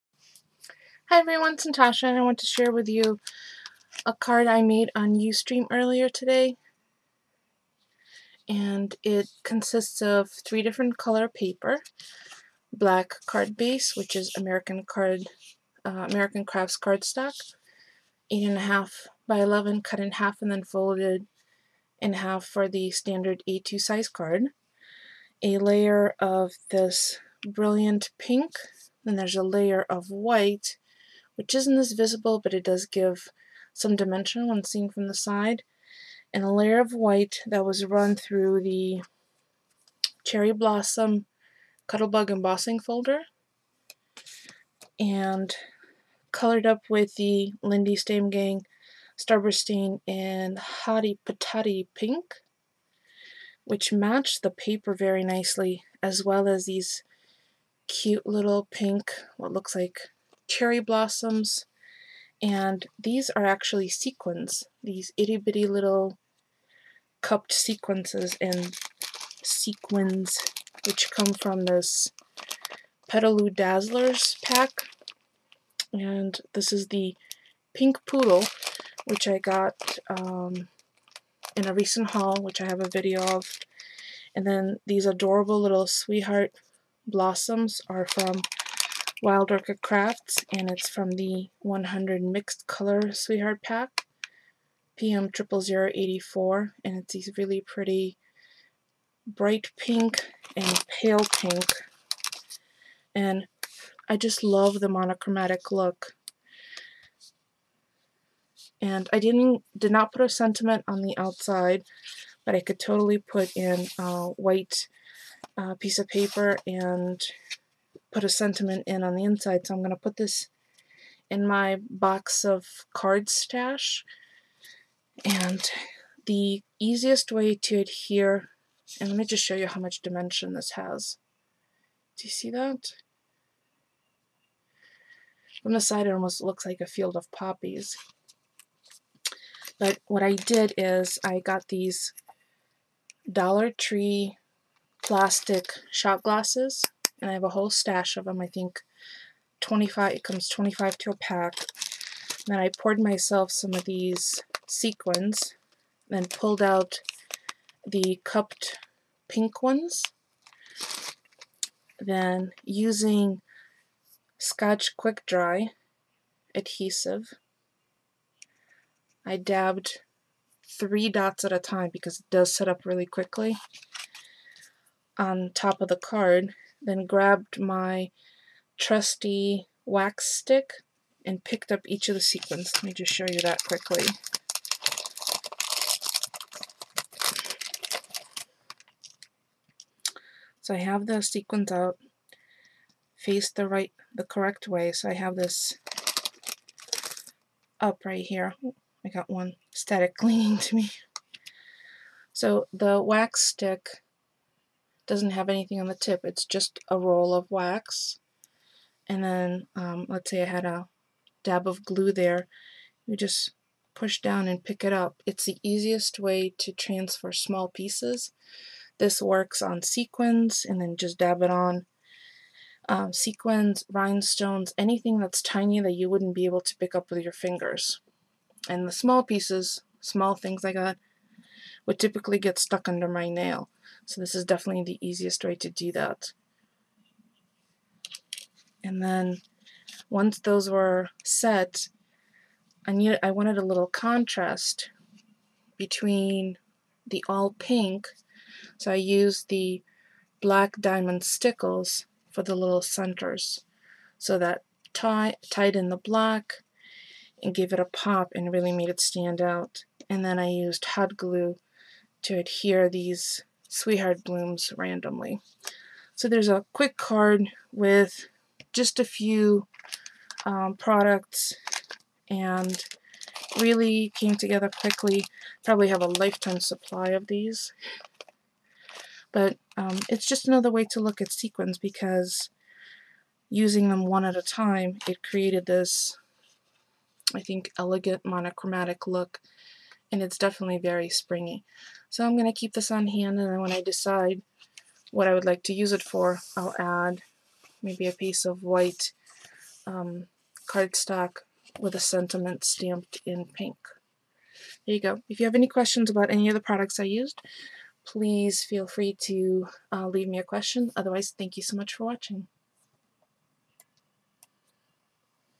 Petaloo Dazzlers Card
National/World Craft Month Video #17
A simple 3 color card showcasing Petaloo Dazzlers (sequins) and Wild Orchid Crafts Sweetheart Blooms in pink.